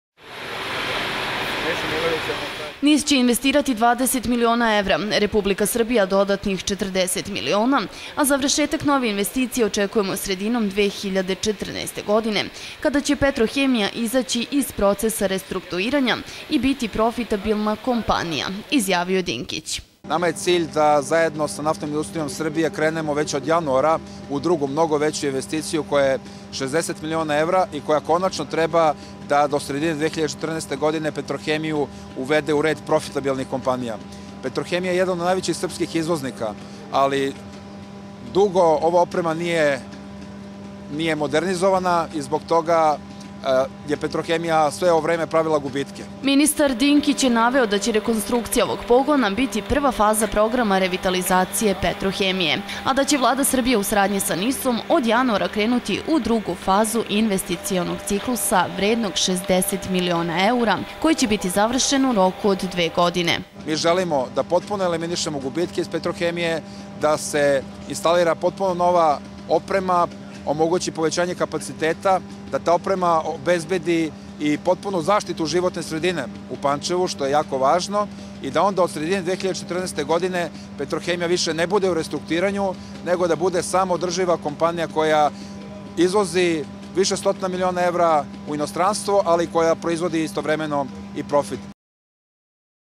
TV KANAL 9, NOVI SAD: Nova ulaganja u Petrohemiju
(VIDEO K9) Država će od januara sa Naftnom industrijom Srbije  pokrenuti  zajednička ulaganja  u Petrohemiji, vrednu 60 miliona evra, koja će biti zavrsena 2014. godine kada će ta kompanija izaći iz procesa restruktuiranja i postati profitabilna.